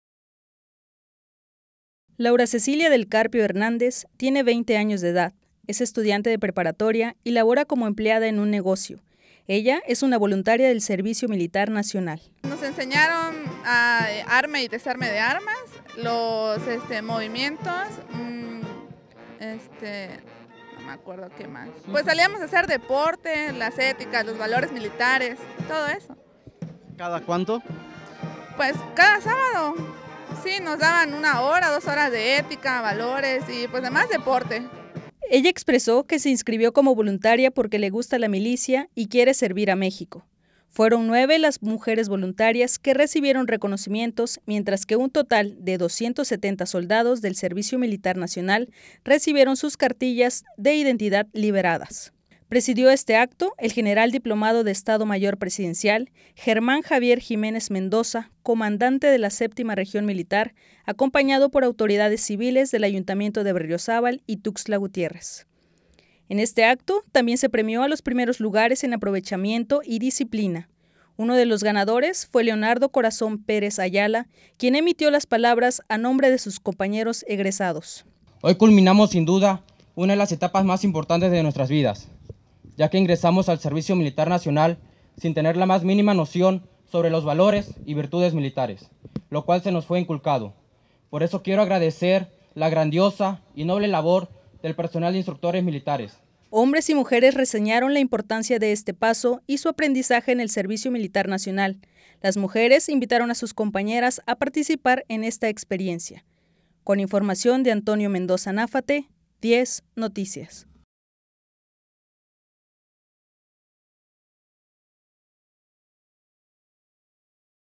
Reconocen a mujeres voluntarias que egresan del Servicio Militar Nacional
Presidió este acto el General Diplomado de Estado Mayor Presidencial, Germán Javier Jiménez Mendoza, comandante de la 7ª región militar, acompañado por autoridades civiles del ayuntamiento de Berriozábal y Tuxtla Gutiérrez.
También se premió a los primeros lugares en aprovechamiento y disciplina, uno de los ganadores fue Leonardo Pérez Ayala, quien emitió las palabras a nombre de sus compañeros egresados.